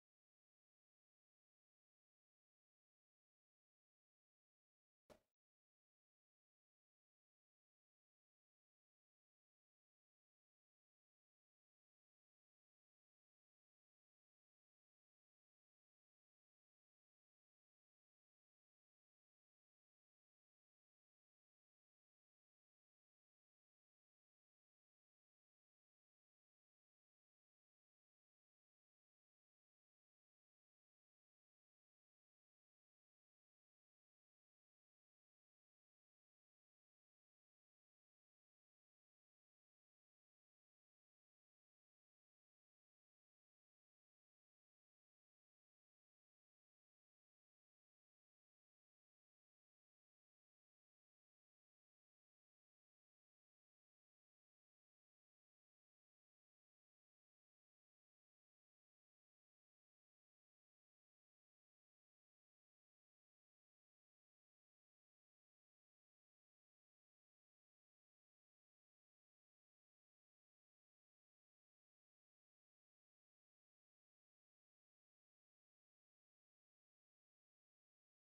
Grinding Tungsten electrodes with AutoGrind
The easy way to grind Tungsten electrodes with the AutoGrind on Ultima-Tig from Inelco Grinders.